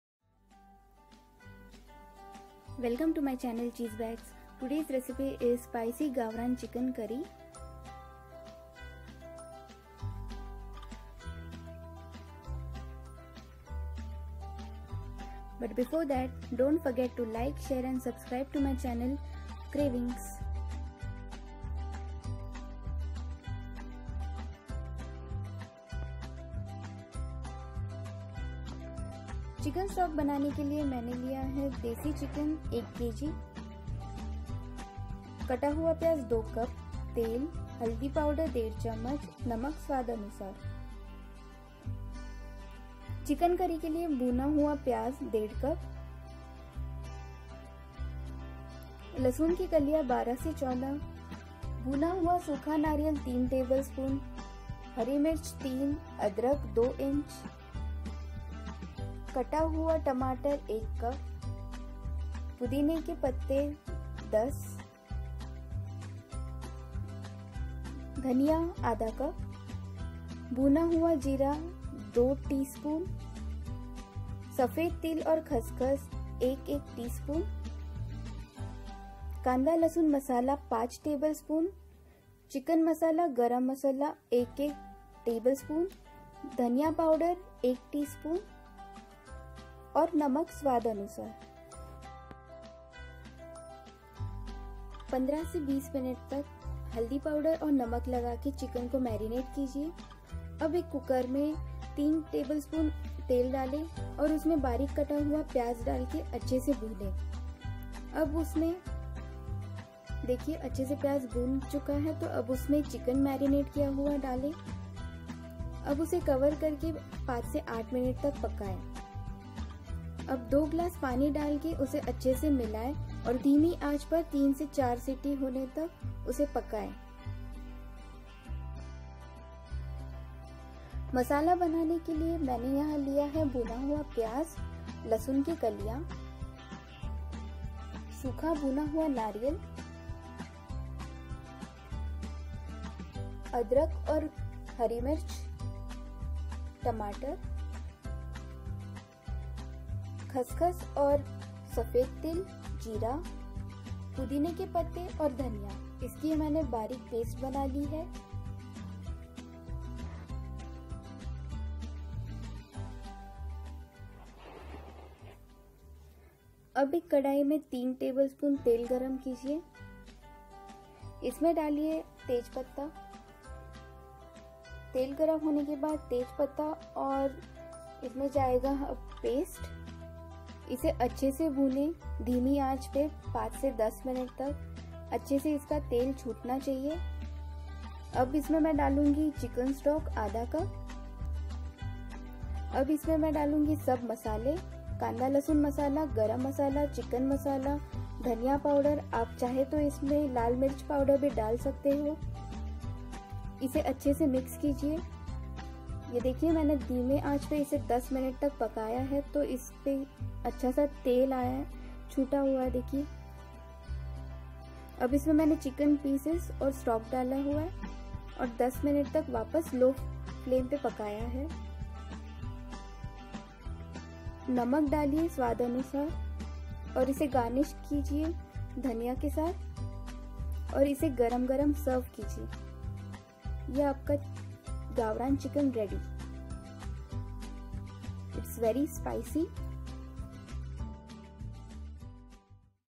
Spicy Gavraan Chicken Curry | झणझणीत गावरान चिकन रस्सा
Spicy Gavran Chicken Curry |झणझणीत गावरान चिकन रस्सा

Learn to make traditional Maharashtrian style chicken curry from country chicken. It's very tasty and spicy curry, can be served with Bhakri/Chapati/Roti and rice.

Note:If you want you can also add Red chilli powder.

Ingredients to make Chicken Stock / चिकन स्टॉक बनाने के लिए सामग्री

Country chicken/Desi Murg/ गावरान चिकन  - 1kg
Finely Chopped onions/ कटा हुआ प्याज- 2 cups
Turmeric powder/ हल्दी पाउडर - 1½tsp
Oil/तेल - 3tbsp
Salt to Taste /नमक स्वादअनुसार

Ingredients to make Chicken Curry /चिकन करी बनाने की सामग्री

Roasted Chopped Onions /भुना हुआ कटा हुआ प्याज - 1½cup
Chopped Tomato/कटे हुए टमाटर - 1 cup
Chopped Coriander leaves/धनिया - 1/2cup
Mint leaves/ पुदीने की पत्तियां - 10
Ginger/ अदरक - 2inch
Green chillies/हरी मिर्च  - 3
Garlic cloves/लहसुन - 12-14
Roasted Cumin seeds/भुना जीरा - 2tsp
Roasted Sesame seeds/ भुना हुआ तिल - 1tsp
Roasted Poppy seeds/भुना हुआ खसखस - 1tsp
Garam masala/गरम मसाला - 1tsp
Chicken masala/चिकन मसाला
- 2tsp
Onion Garlic Masala/Kanda Lasun Chutney/प्याज लहसुन मसाला - 5tbsp
Coriander Powder/धनिया पाउडर
- 1tsp
Bay leaves/ तेज पत्ता - 3

Happy Cooking!